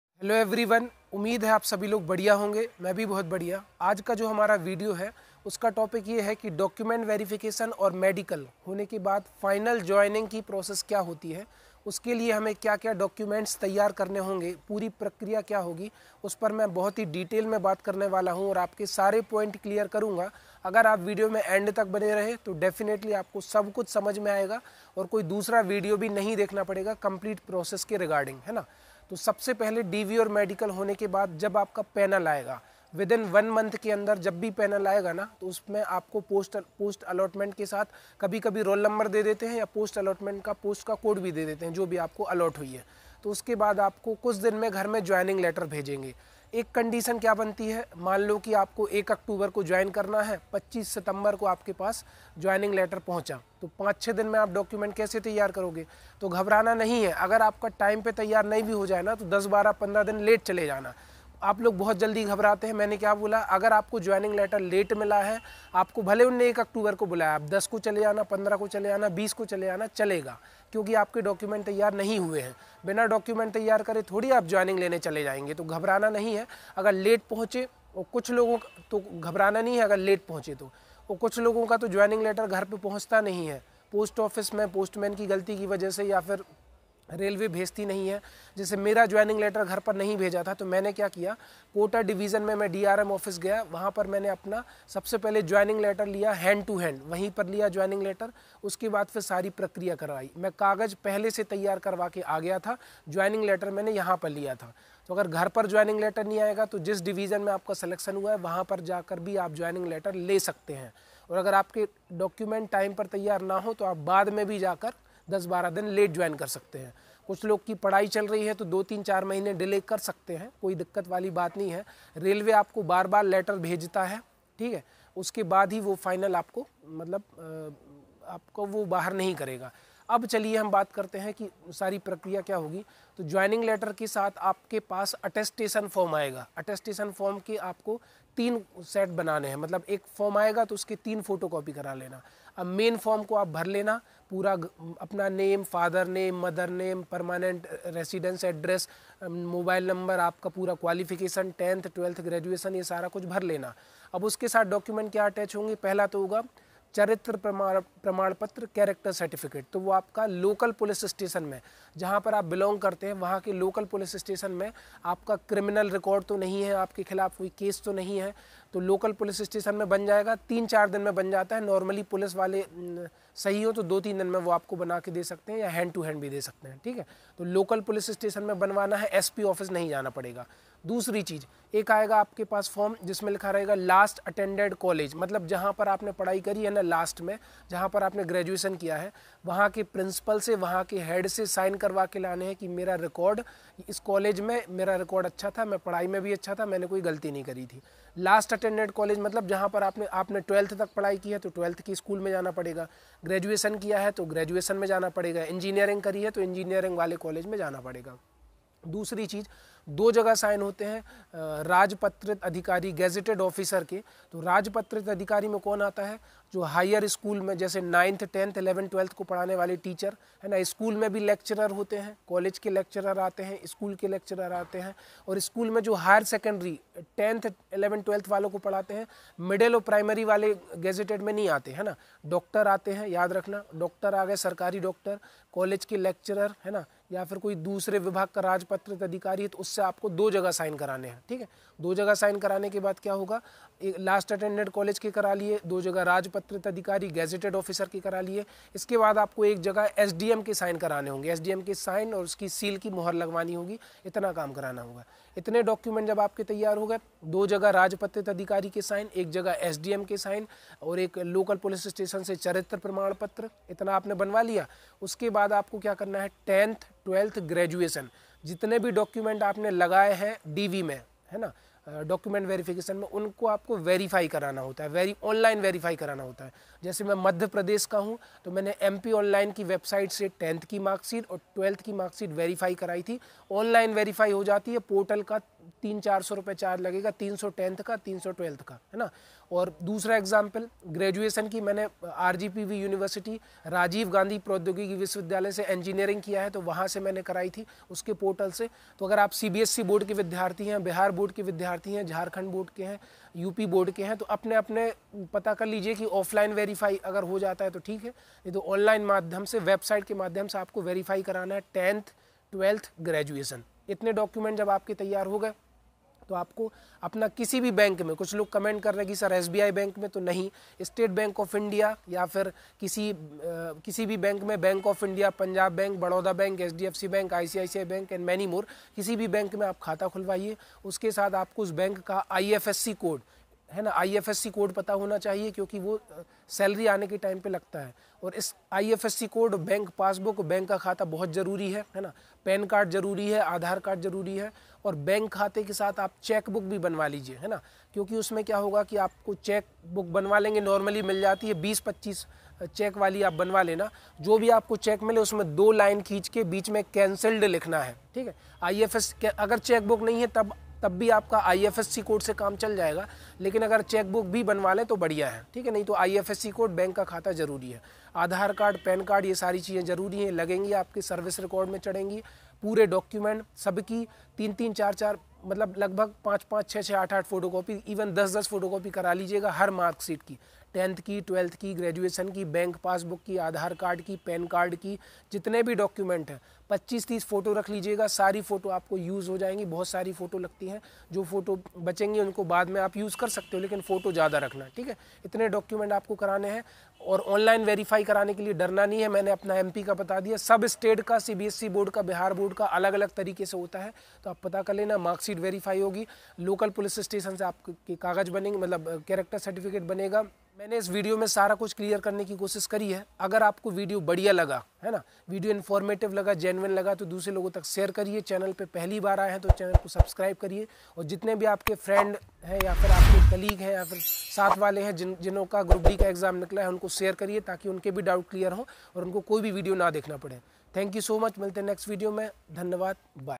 railway group d complete joining process
rrc group d complete joining process |
group d me joining me kya document lagte hai |